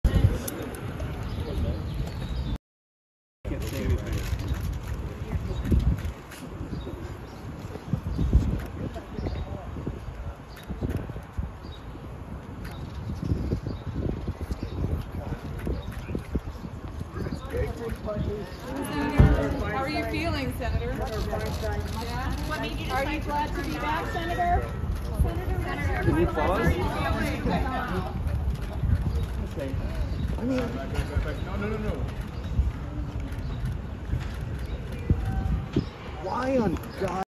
Sen. Feinstein arrives back on Capitol Hill after illness
RESTRICTION SUMMARY:

ASSOCIATED PRESS
Washington DC - 10 May 2023
1. Sen. Dianne Feinstein car arriving
++BLACK FRAMES
2. Senator gets out of car into wheelchair with Chuck Schumer looking on

STORYLINE:
California Democratic Senator Dianne Feinstein has returned to Capitol Hill after a lengthy time away due to illness.

Feinstein is 89 and had been battling shingles.

She returned to Washington in a wheelchair and was greeted by Senate Majority leader Chuck Schumer.

Feinstein had been absent for more than two months. During that time the oldest member of Congress faced calls from within her own party to resign.

She announced in early March that she had been hospitalized in San Francisco and was being treated for a case of shingles. But an expected return later that month never happened.

Few details emerged on Feinstein's condition, and some Democrats openly complained that her lengthy absence was compromising the Democratic agenda in the Senate, including slowing the push to confirm President Joe Biden's judicial nominees. Some in the House urged her to step down.

Earlier this month, Feinstein said in a statement that “there has been no slowdown.”